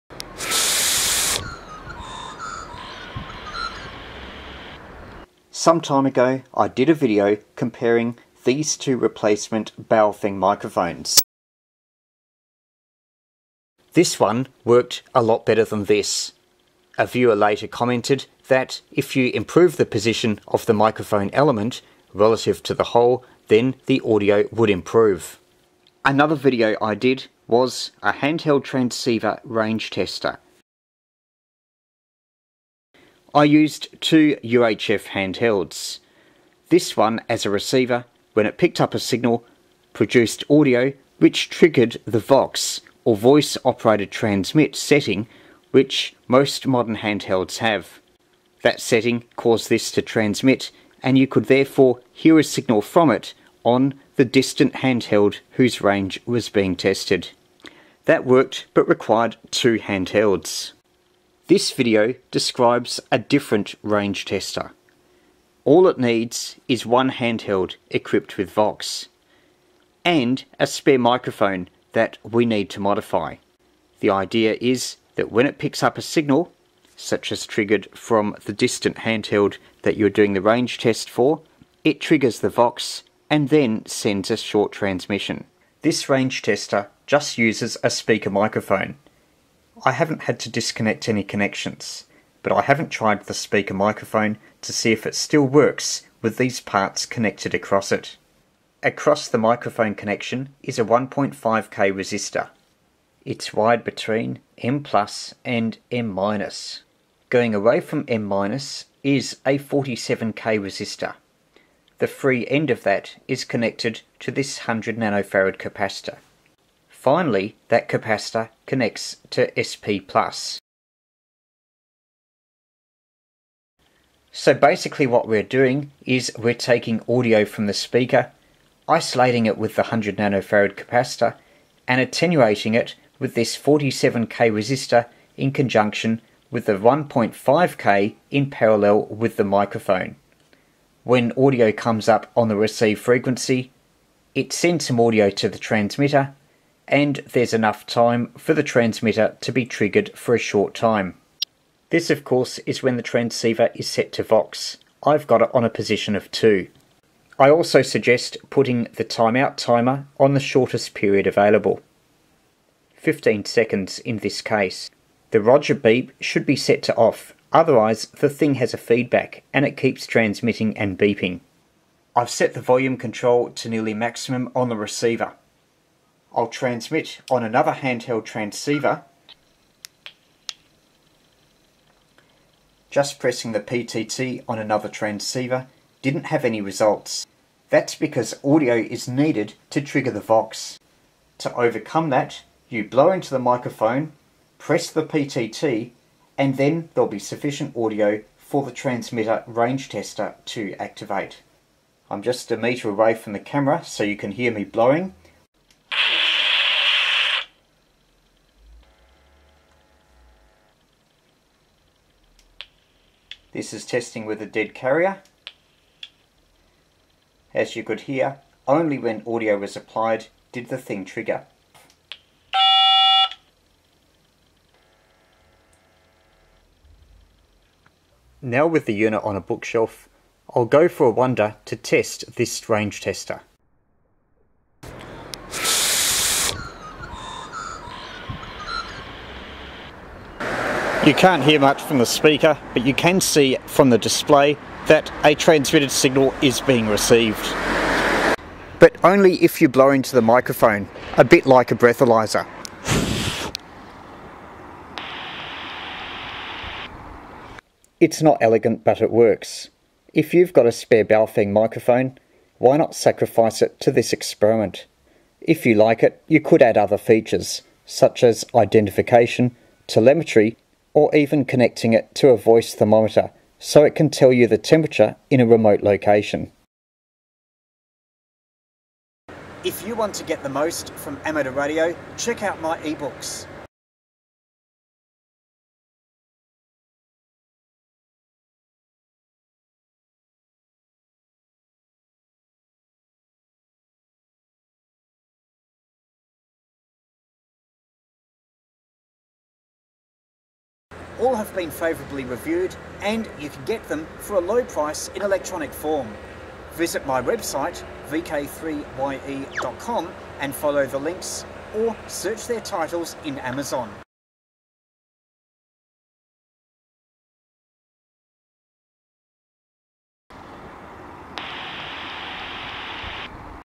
VHF/UHF range tester uses a single Baofeng handheld transceiver
A range tester using a single Baofeng handheld VHF/UHF FM transceiver.  See how far your handheld can go. Requires no internal modifications.  Uses VOX function to generate 'tail' when modulated carrier is applied.

Don't have a Baofeng?